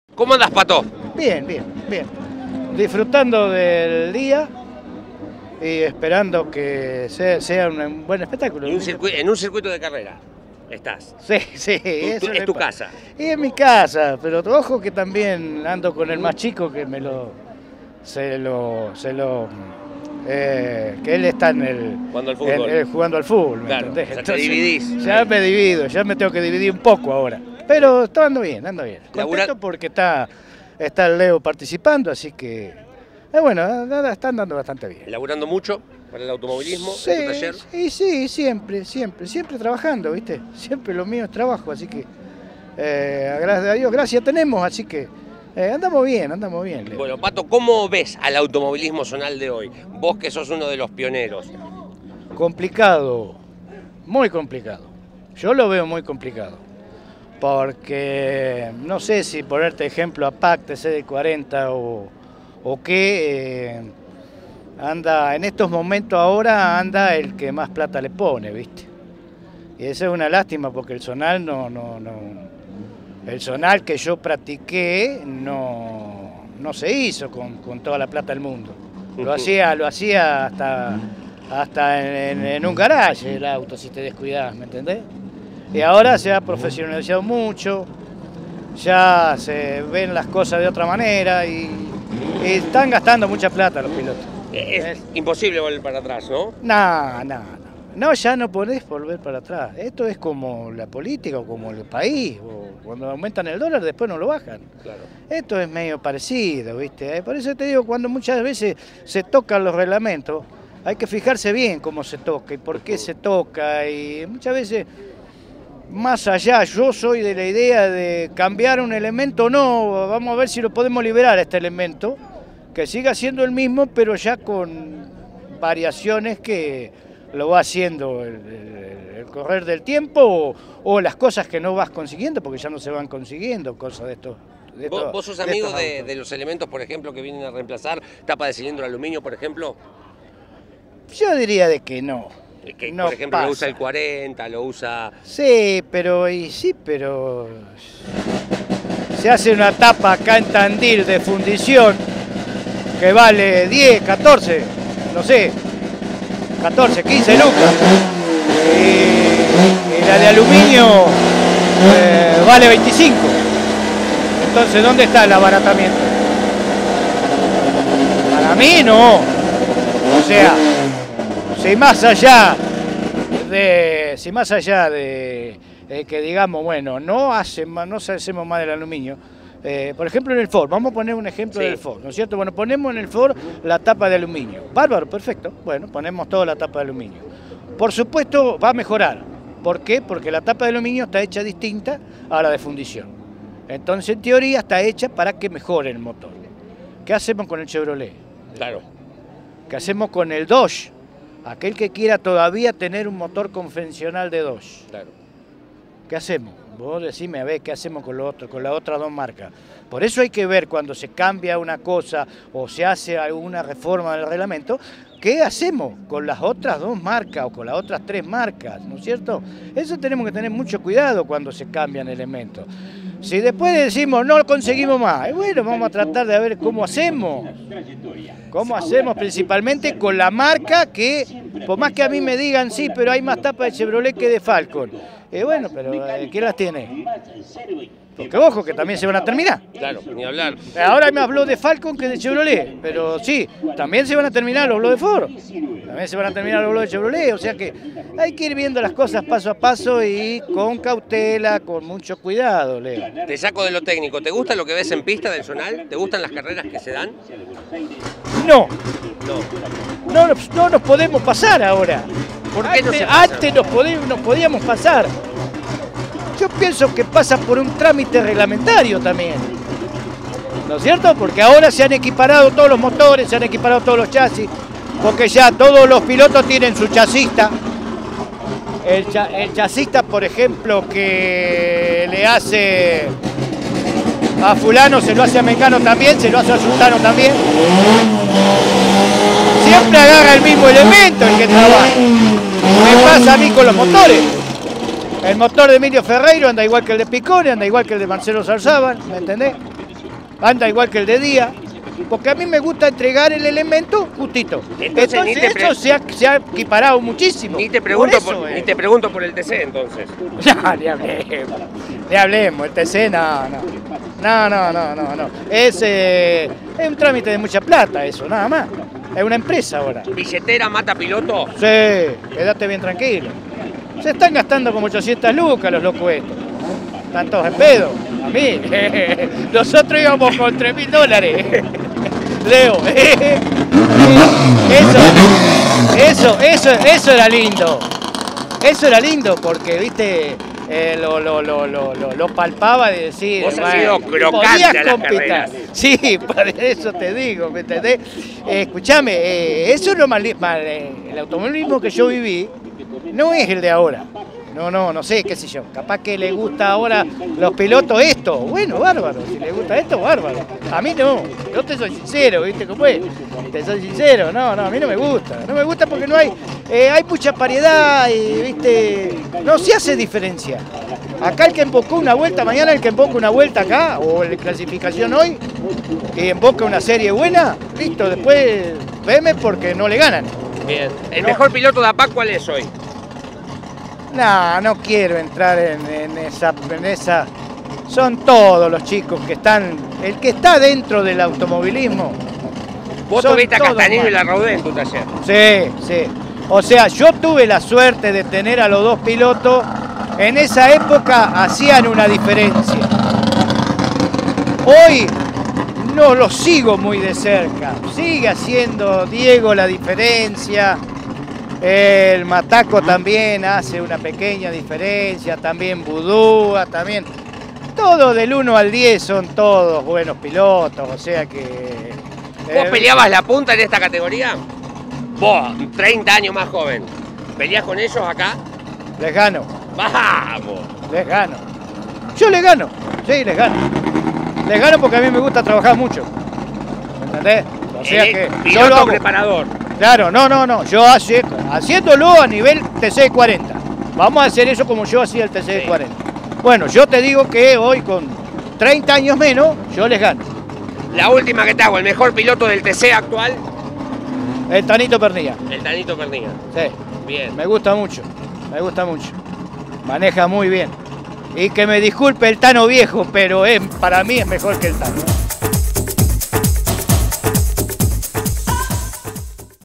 Entrevista: Jorge Politano
Un diálogo imperdible de Leonardo Moreno Labat con el legendario piloto y preparador serrano.